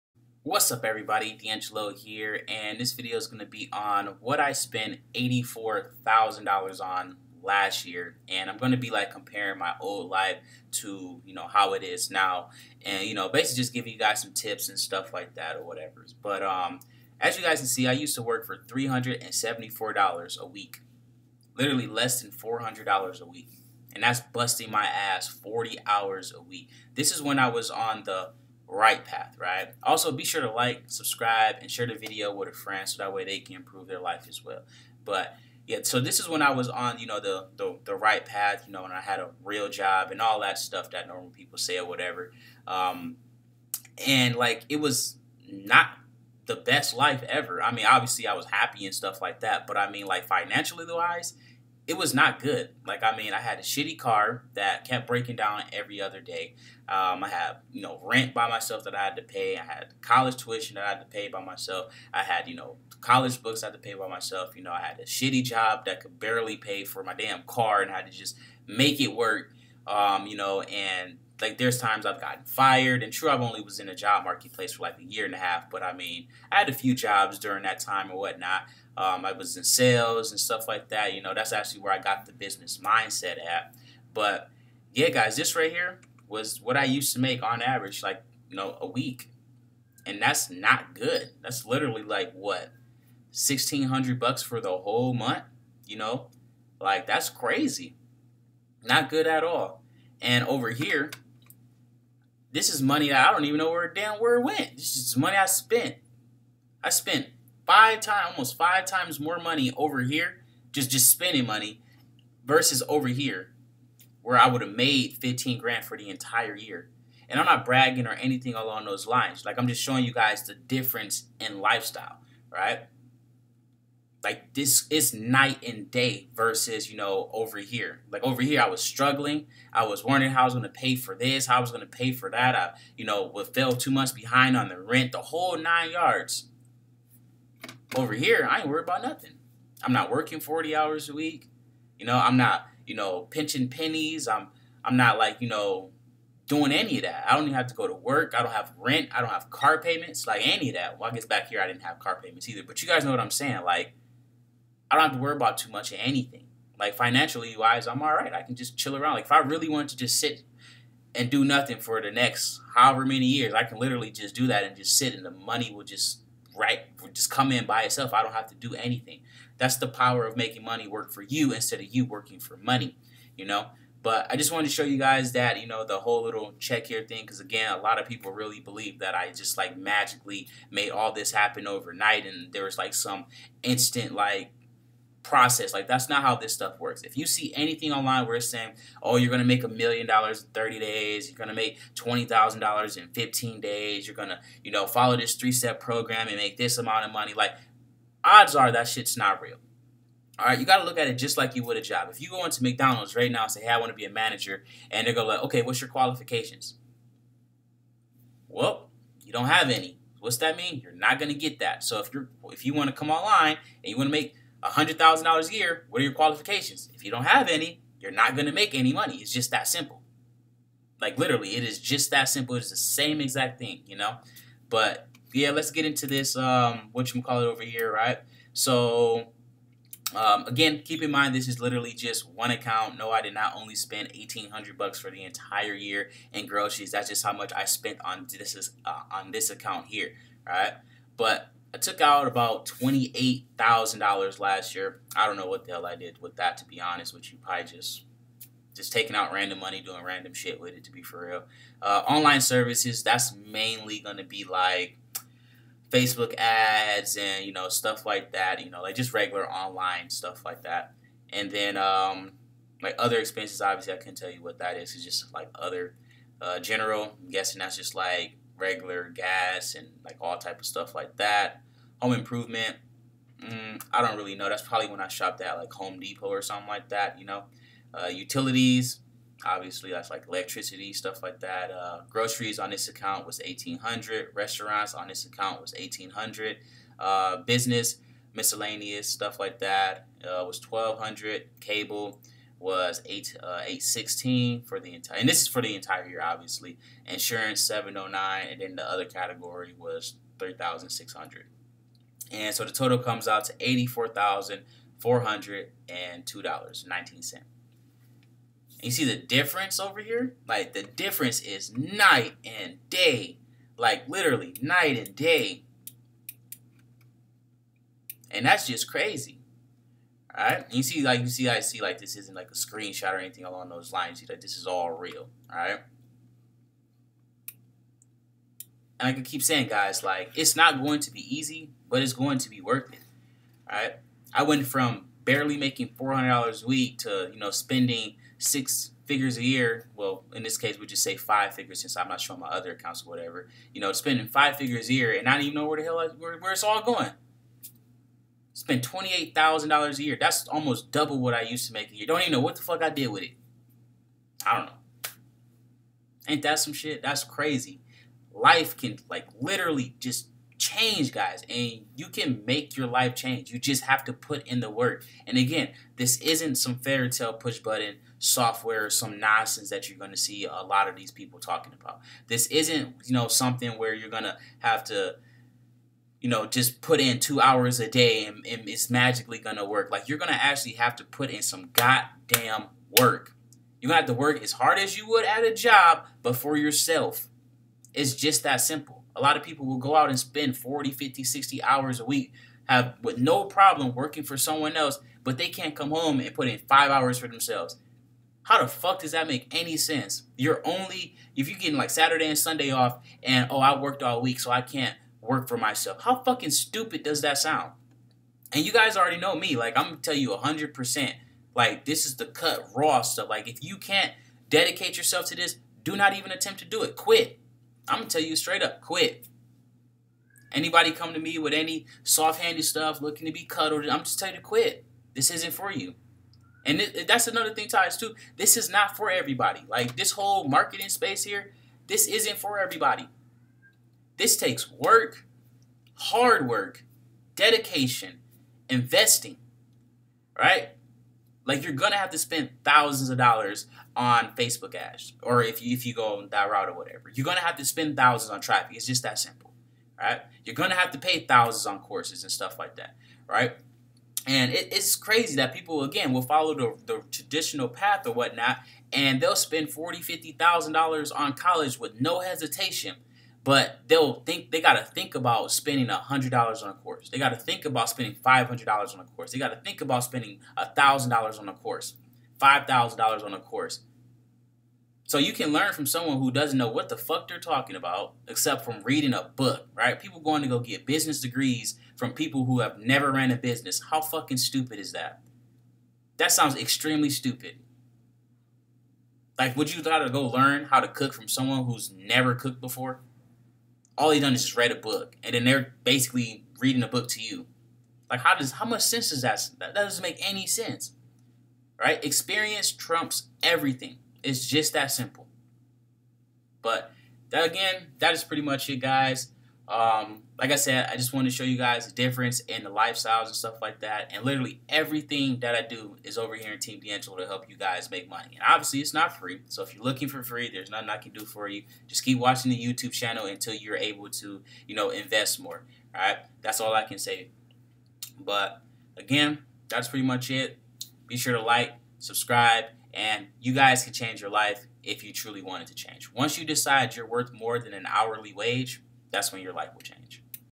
I Spent $84,000 Last Year In 1 Account - Affiliate Marketing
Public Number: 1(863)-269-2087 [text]

I used to be a broke college student working for $400 a week, affiliate marketing has completely changed my life.

CPA Marketing, click bank, direct sales, flippa, flipping websites, flipping apps, passive income, etc. I have done a lot of stuff online, and it took time to get where I am now, and I still have a long way to go buy it's better than day 1 for sure.

Marketing is tough, but you just have to get started. Being broke was a good lesson, one I hope I never have to learn again.

Your life can change too when you realize you are worth more than an hourly wage.

College wasn't for me, but it might be for you.

Be sure to check out the free training and my success below ⤵